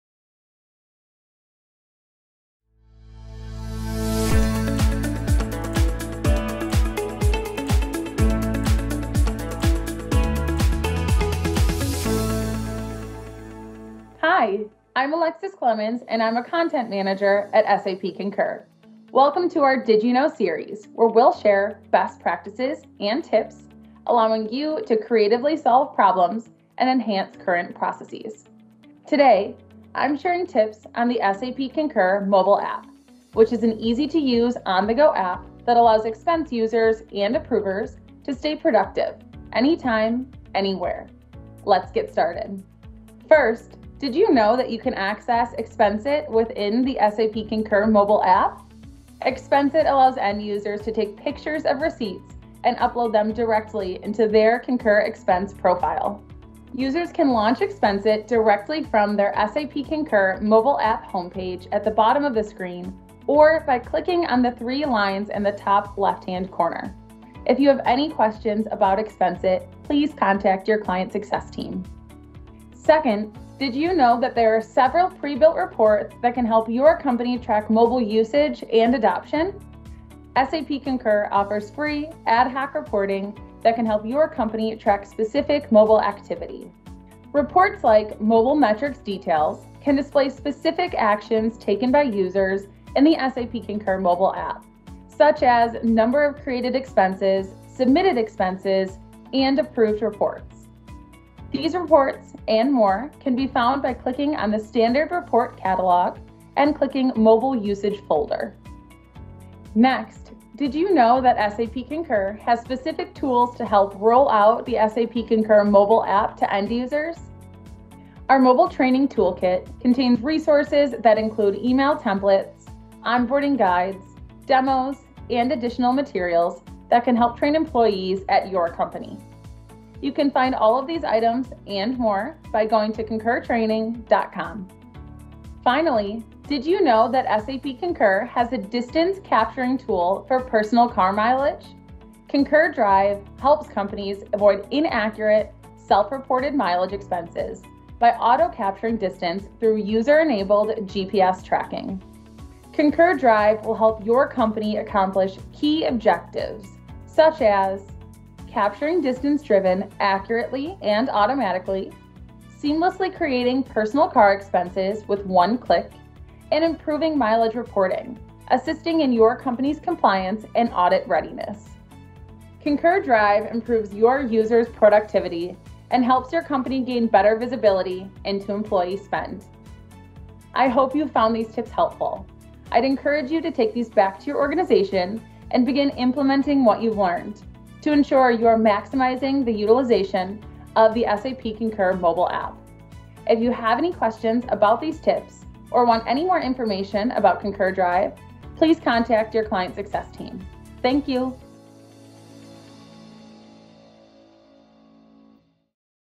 Did You Know... Tips & Tricks for the SAP Concur Mobile App
Welcome to the “Did You Know…” series, where we’ll share best practices and tips allowing you to creatively solve problems and enhance current processes. Today we are sharing tips on utilizing the SAP Concur Mobile App to help your teams operate more efficiently.